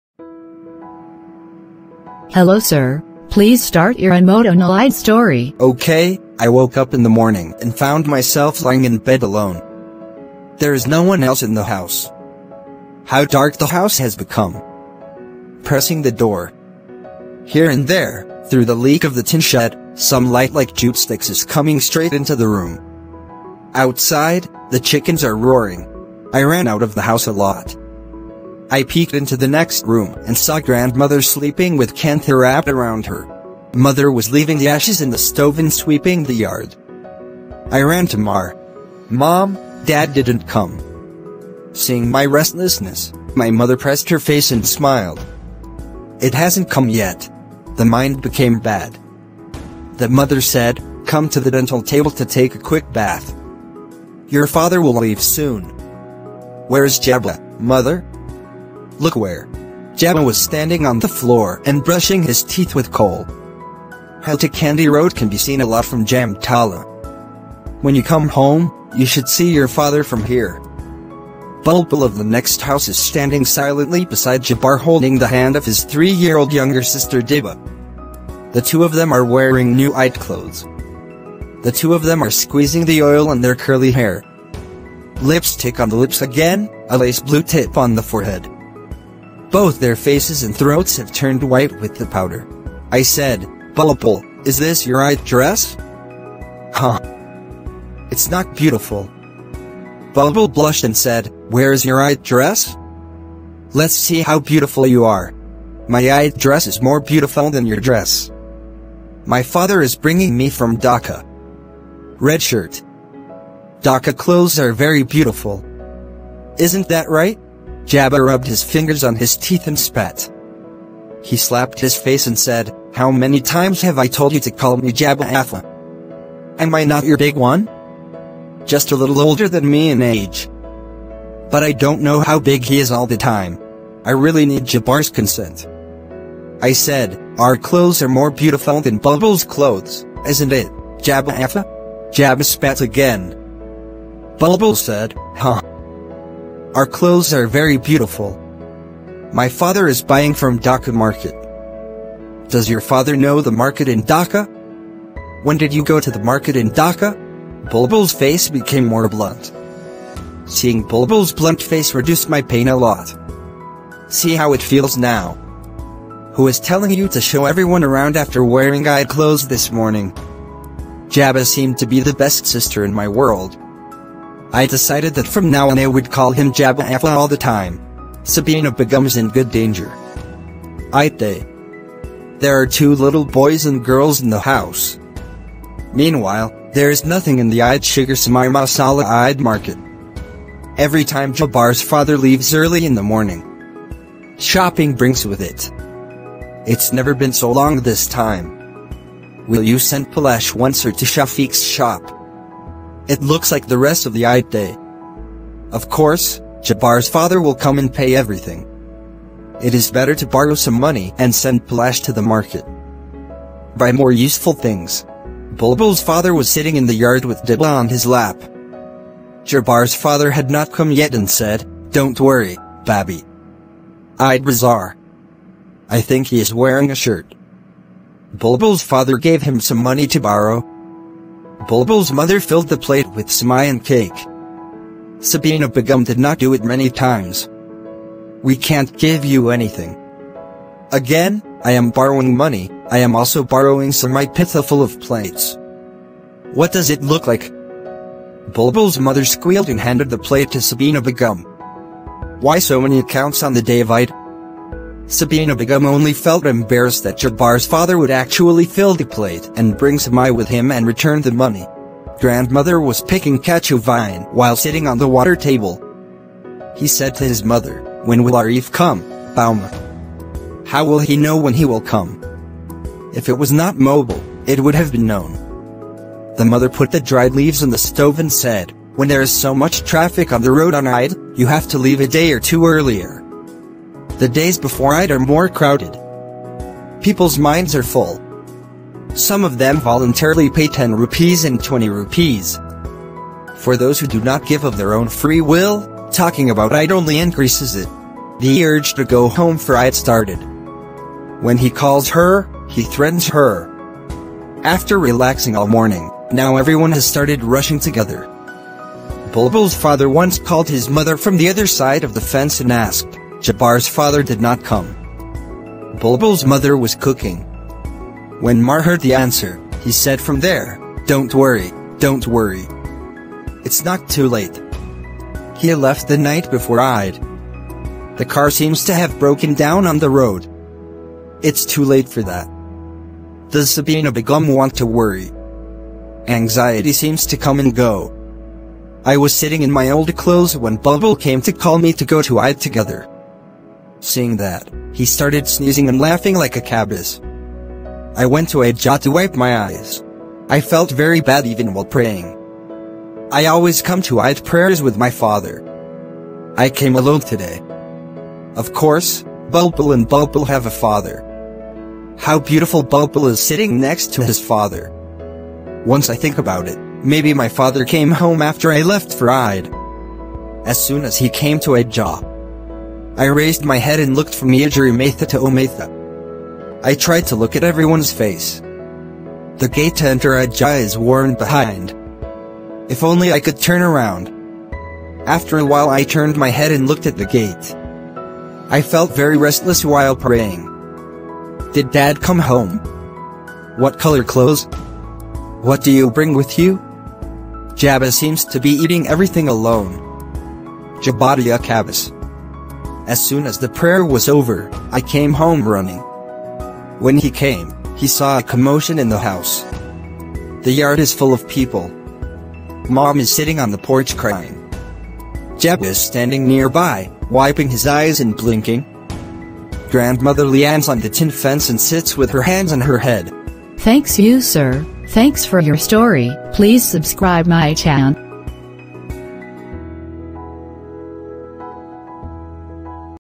father did not come home | Islamic Kids Cartoon | 3D Animation | Eid-ul-Adha | HD | 2020
FAIZAN-E-MADINA - GLOBAL ISLAMIC CENTER (AALAMI MADANI MARKAZ)
I woke up in the morning and found myself lying in bed alone. There is no one else in the house. How dark the house has become. Pressing the door. Here and there, through the leak of the tin shed, some light like jute sticks is coming straight into the room. Outside, the chickens are roaring.

video made by-plotagan story